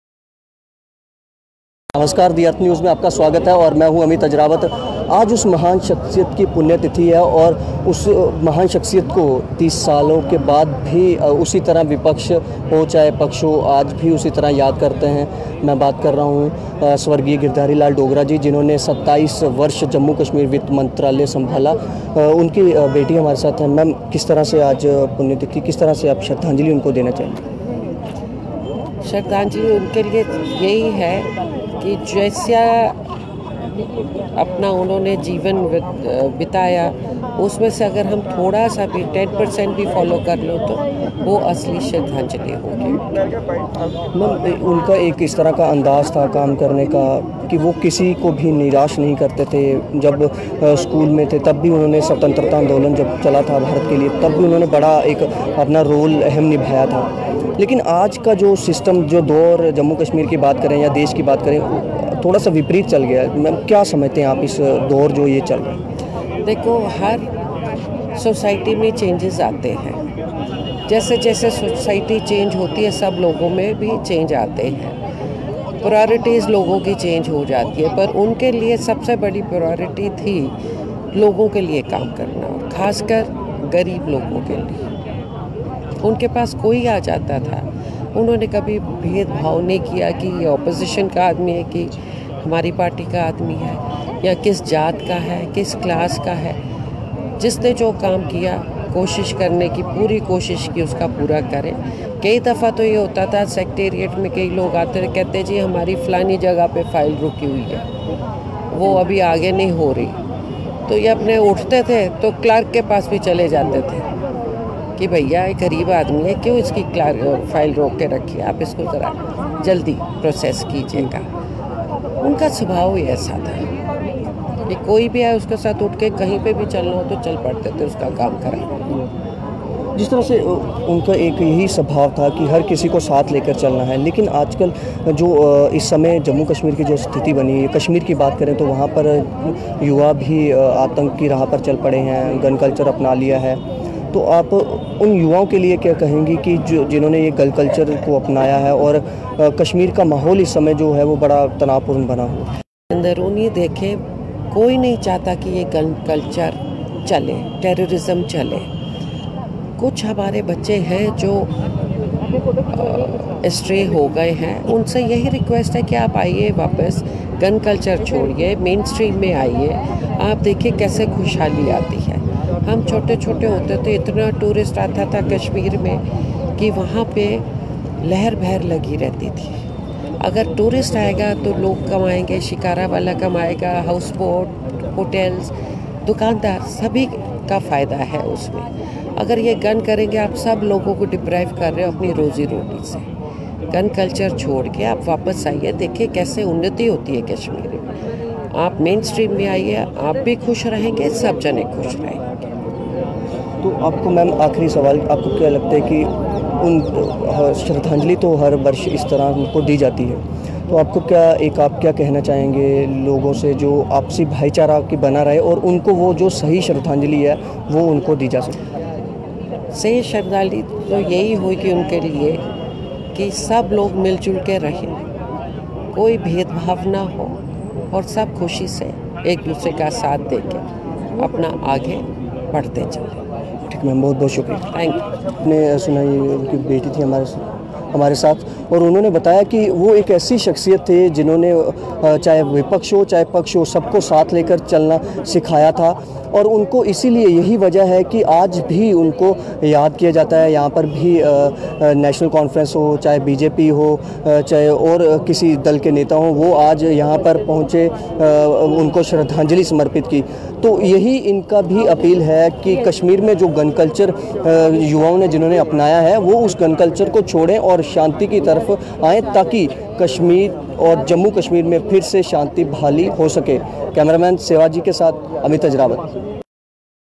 No one wants this Gun Culture to continue
No one wants this Gun Culture to continue: Daughter of Gridhari Lal Dogra